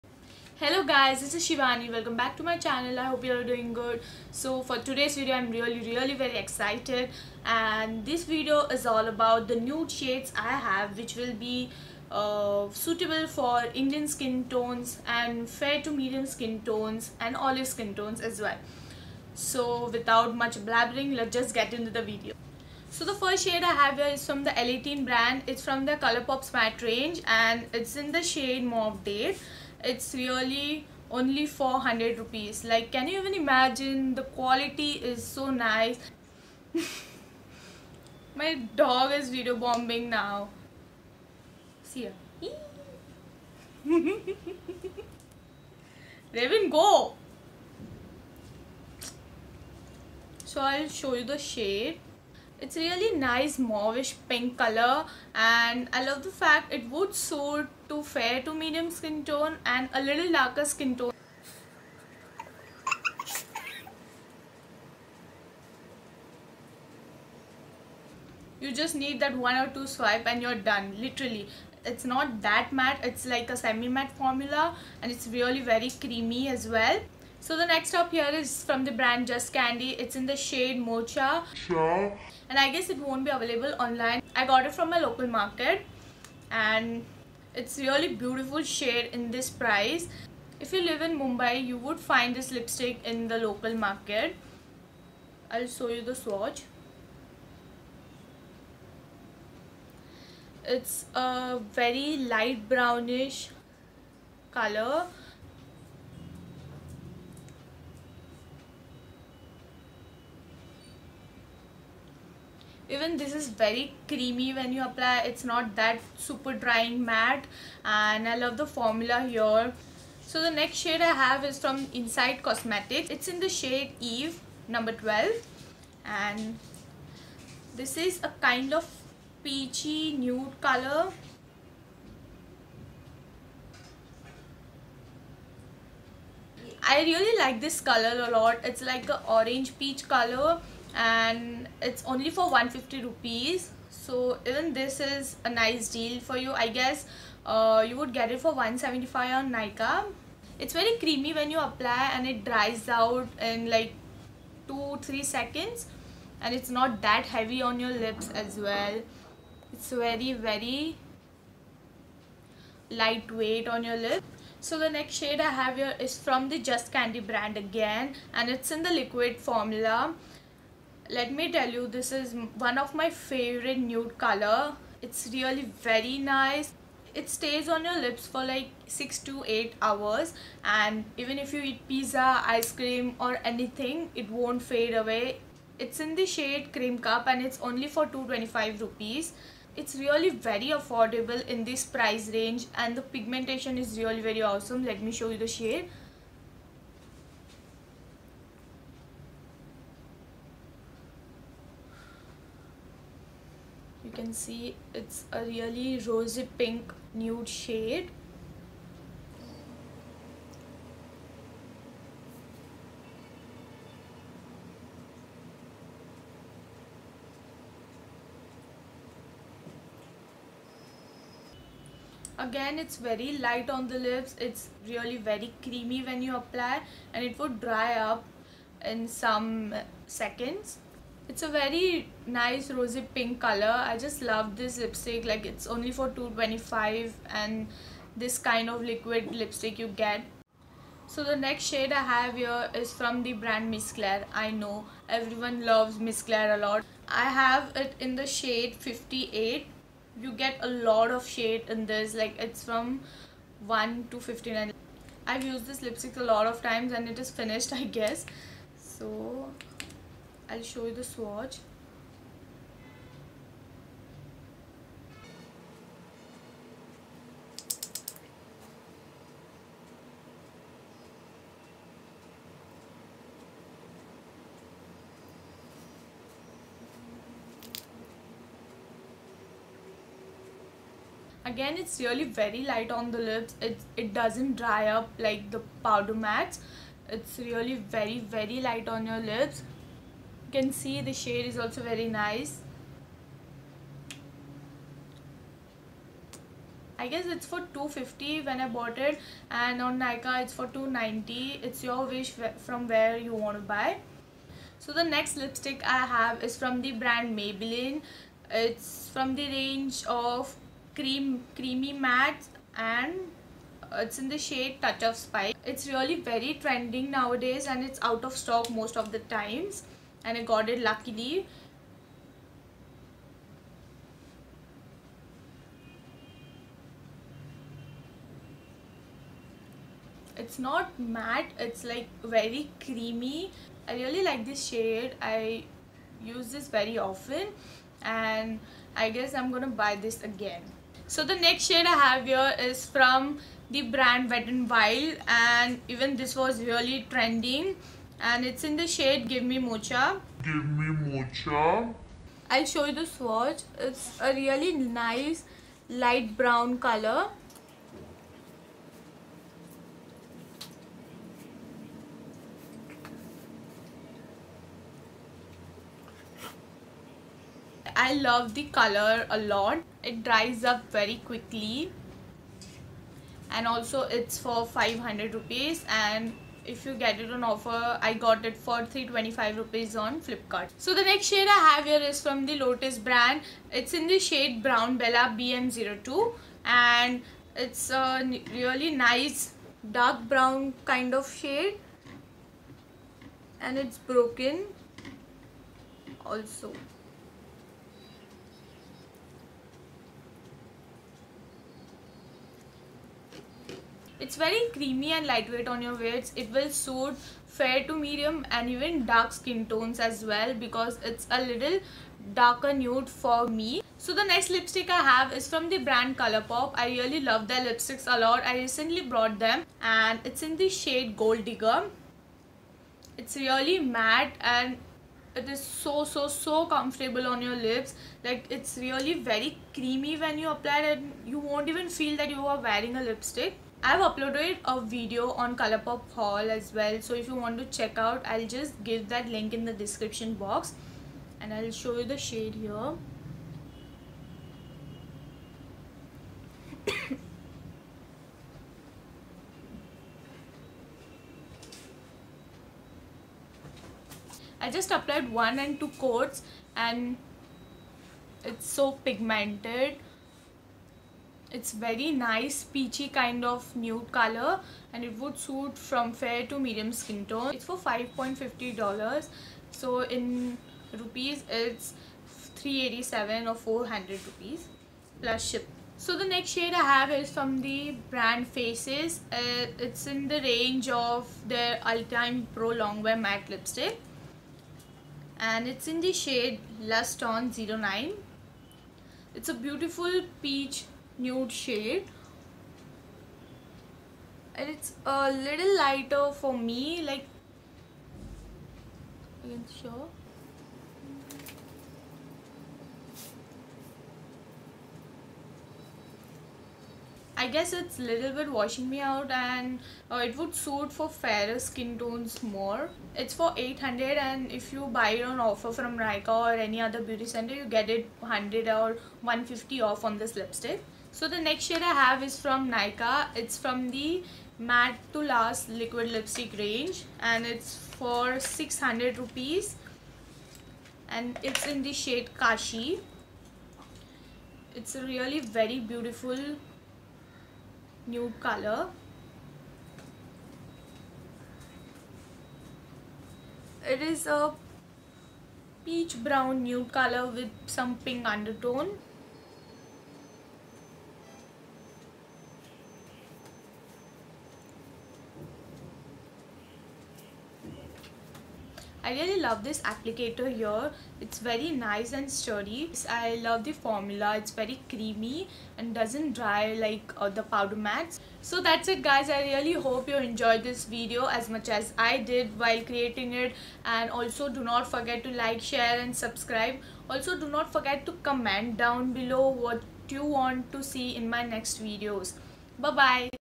TOP NUDE LIPSTICKS FOR INDIAN / MEDIUM / OLIVE SKINTONE | SHIWANI KHADE
HELLO EVERYONE!
IM BACK WITH ANOTHER VIDEO.
ELLE 18- MAUVE DATE RS. 100
JUST CANDY- MOCHA RS.150
INSIGHT COSMETICS- EVE 12 RS.175
JUST CANDY- CREME CUP RS.225
MAYBELLINE- TOUCH OF SPICE RS.300
FACES CANADA- LUST ON 09 RS.800
NYKAA- KASHI RS.600
WET N WILD- GIVE ME MOCHA RS.500
LOTUS- BROWN BELLA RS.650
COLOURPOP- GOAL DIGGER $5.50 RS. 390
MISS CLAIRE- NO. 58 RS. 250

ALSO DO COMMENT ANY IDEAS OR SUGGESTIONS YOU HAVE.